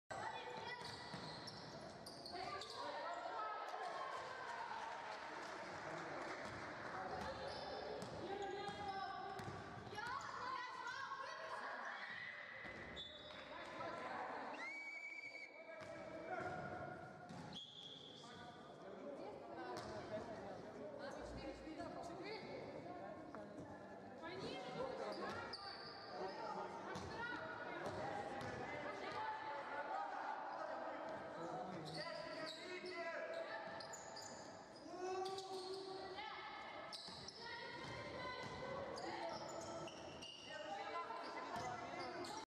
Моменты дня, #3
Мария Попкова, игрок команды NOKIAN-SPB.RU, подвернула ногу во время игры, их соперник - девчонки из Кировской не растерялись.

Всё сделали правильно. Браво!